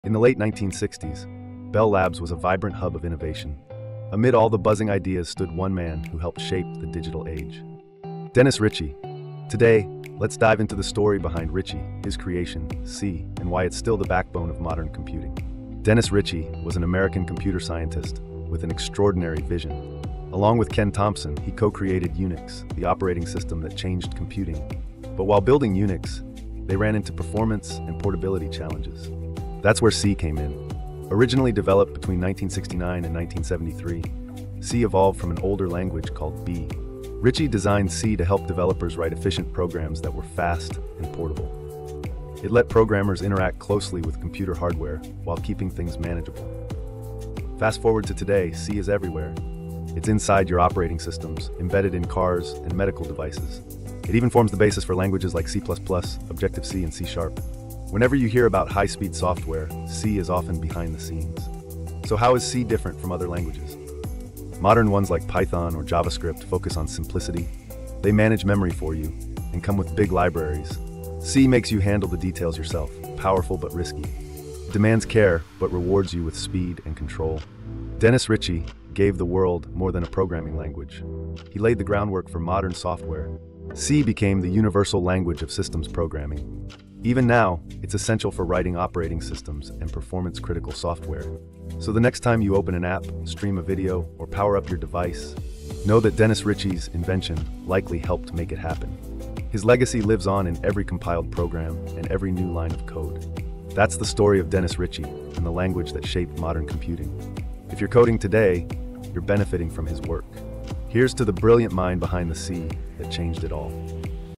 Dennis Ritchie & the Birth of C – A 2-Minute History Lesson
Discover how C programming transformed the world in this quick video from PreCodeCamp.com! 🌍 This video introduces the incredible story of Dennis Richie, the mastermind behind C programming, and demonstrates how his invention became the backbone of modern computing. Whether you're interested in coding, API development, or learning JavaScript and if statements, this video connects the dots between C’s legacy and today’s programming languages.

You’ll explore C’s role in creating powerful software, its influence on high-speed systems like operating systems and embedded devices, and how it paved the way for languages like C++, Objective-C, and JavaScript. Unlike modern languages that simplify tasks, C puts you in control, combining speed with efficiency. If you’re coding today, you're standing on the shoulders of Richie’s groundbreaking work!